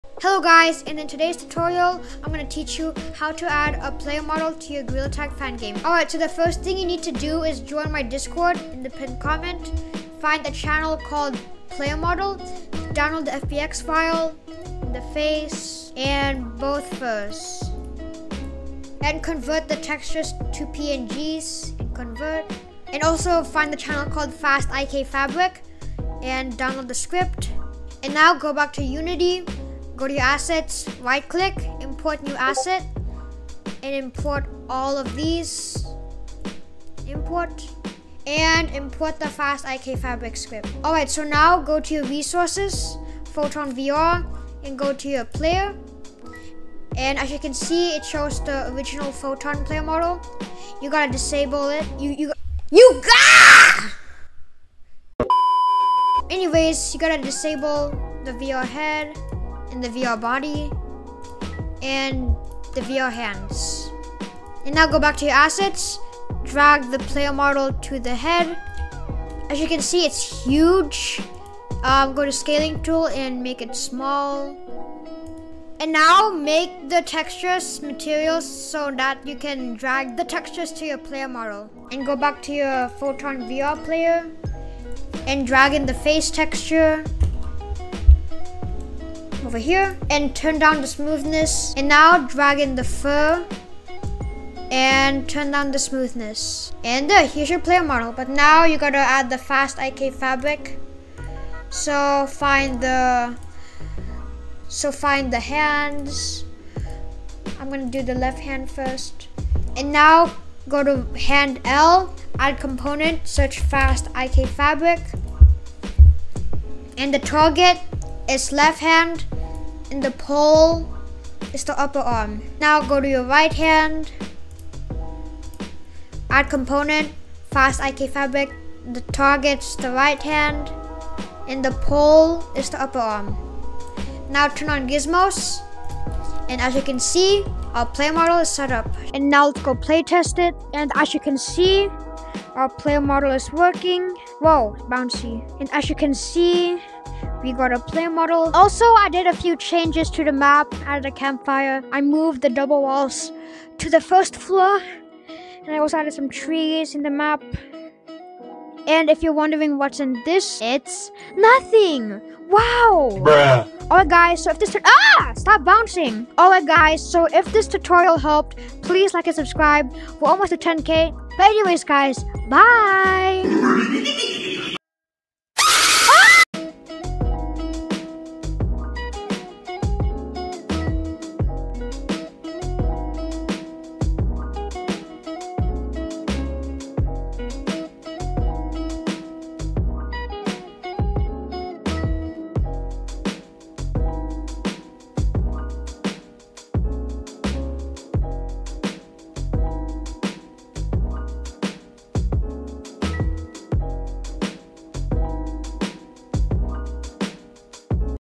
How to Add a Player Model to your Fangame!
Today I will be teaching you how to add a playermodel to your Gorilla Tag Fan Game.